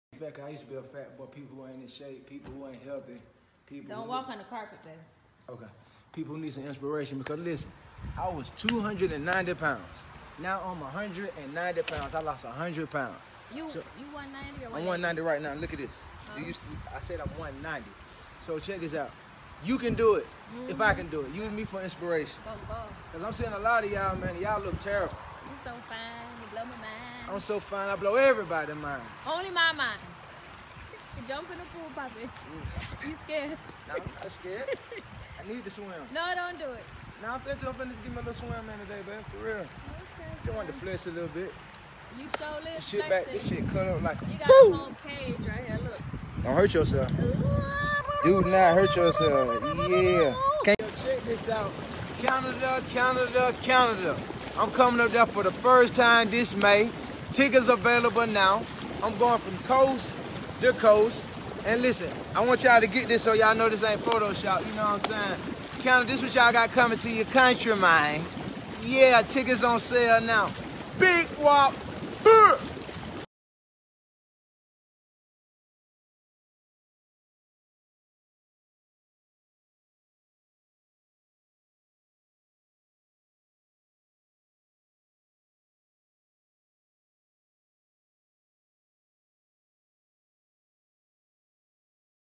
Gucci Man Clone Tries To Trick Us To Think He The Original Gucci
Gucci Man has a new body in 2019
Follow All Urban Central